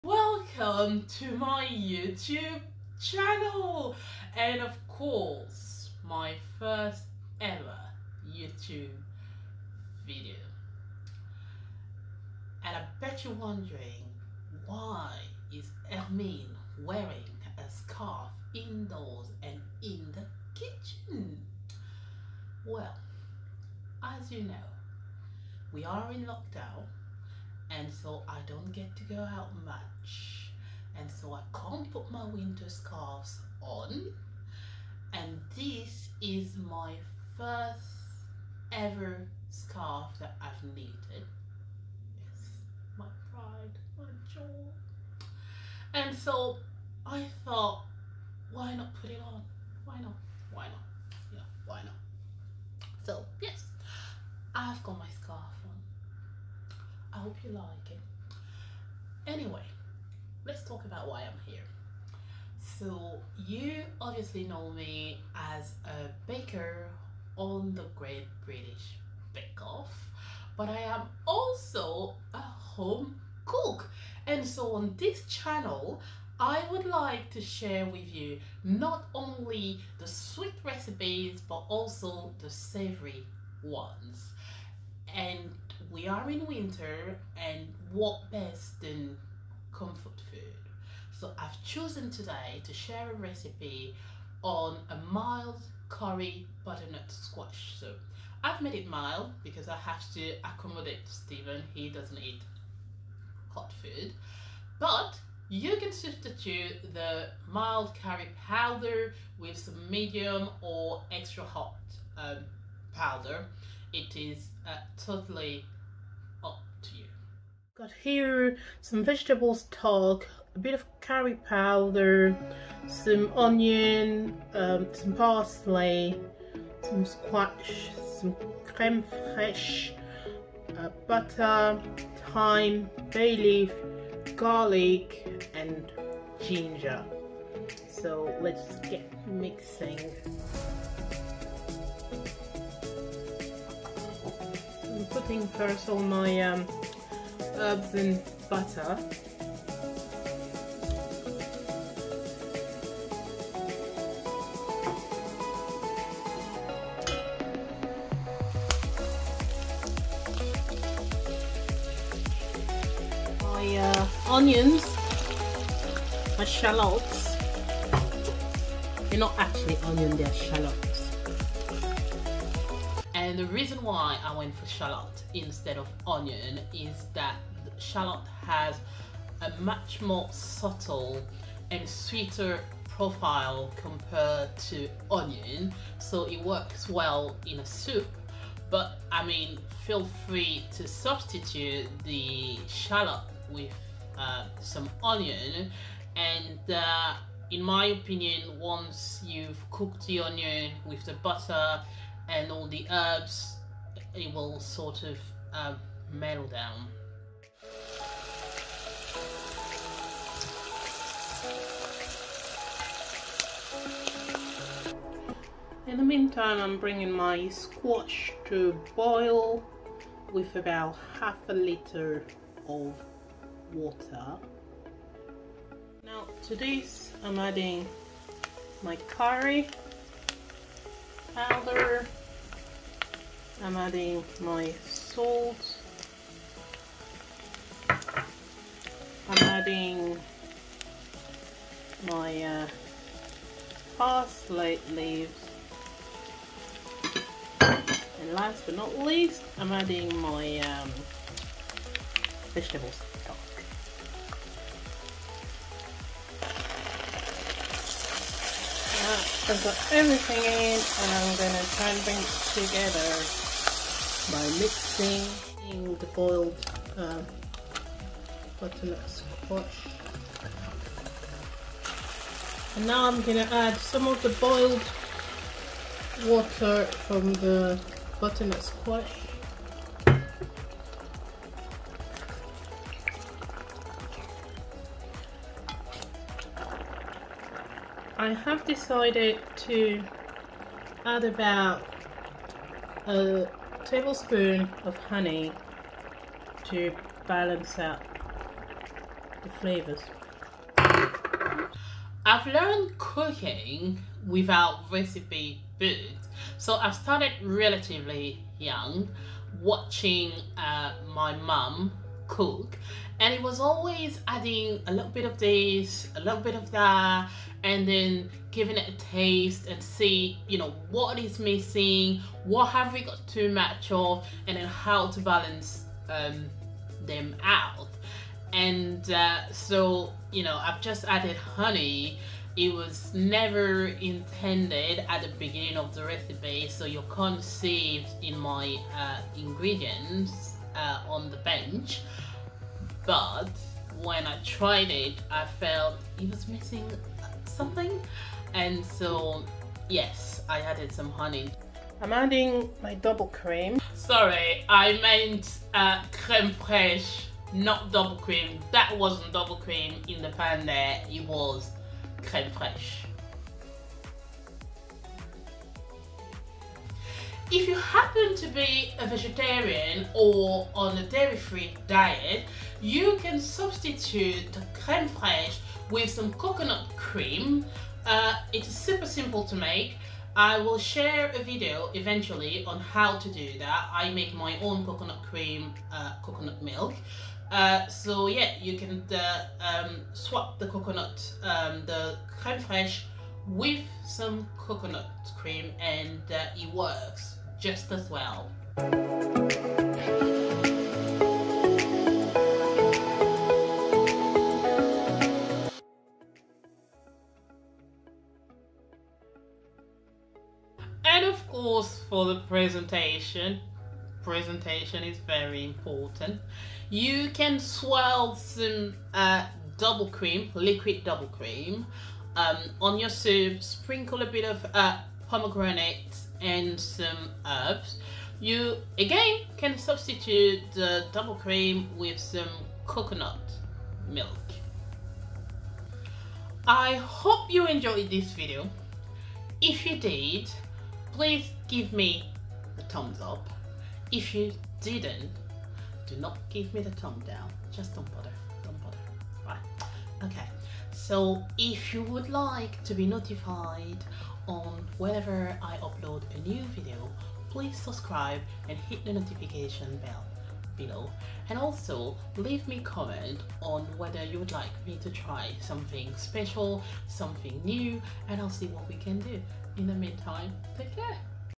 Mild Curry and butternut squash soup 🍲 😋 👌
Ingredients

-50gr of unsulted butter(substitute with olive oil)
-1tsp of chopped ginger
-1tsp of chopped garlic
-1 tbsp of finely chopped parsley
-2 stem of thyme(preferably fresh)
-1 bayleave (preferably fresh)
-1tbsp of curry powder
-1tsp of salt
-1 pot of veg stock
-1tbsp of clear honey
-100gr of shallot
-500gr of butternut squash
-500ml of water (250gr of which to be added to the soup mix after boiling the squash)
-70gr of creme fresh(substitute with coconut cream)

Presentation
-1tsp of finely chopped parsley
-1tsp of pomegranate
-2 tbsp of double cream (substitute with coconut cream)